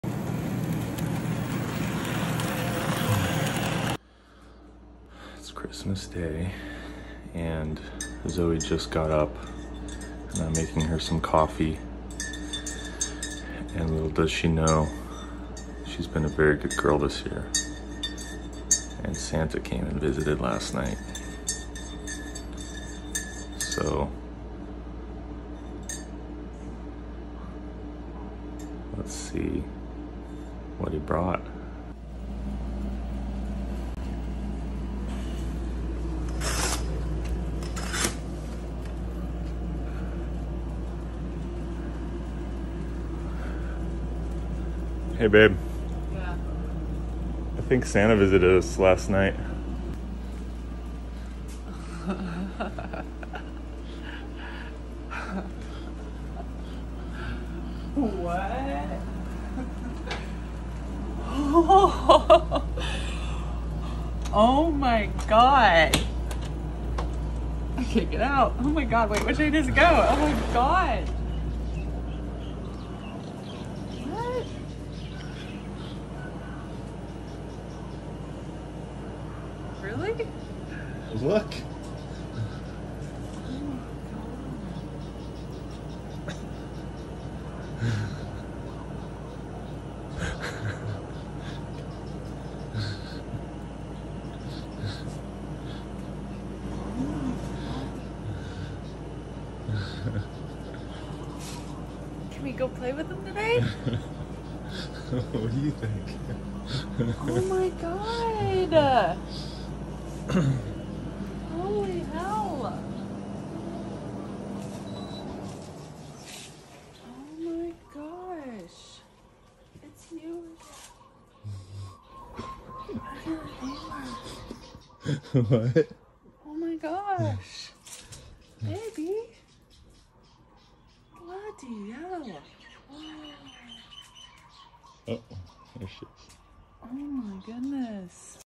Santa Came By Last Night | Specialized Levo SL | Two Fox
Zazz and Papa were very good this year so Santa decided to drop a little something by. We were so excited by these Specialized Levo SL electric bikes we just HAD to go for a ride! Thanks to Grant Miller from Bikeworks Kona for these amazing toys. The Speacialized Levo SL elecgtric bike is a really great electric mountain bike that acts like a regular mountain bike but is only a bit heavier. It really makes climbing a lot more tolerable!